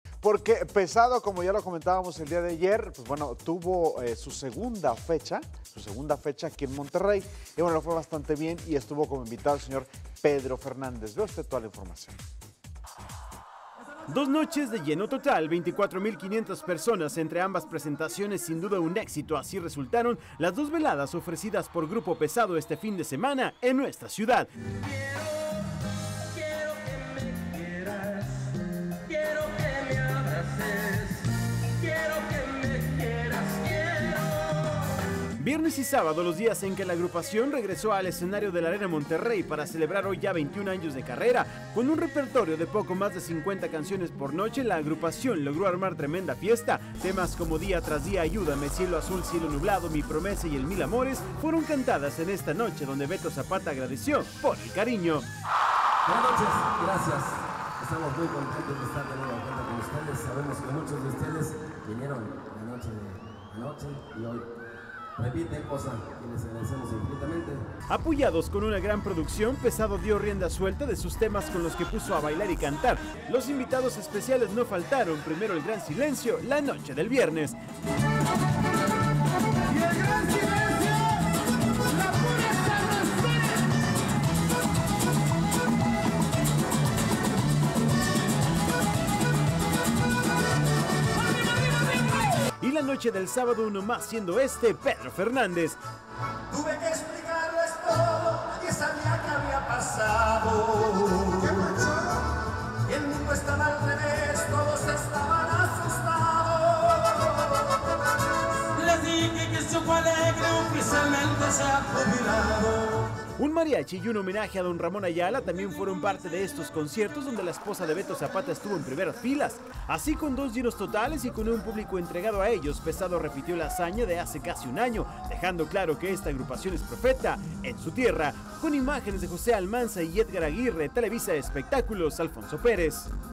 Las Noticias - 24 mil 500 personas disfrutaron de los conciertos de Pesado
Abarrotaron las dos fechas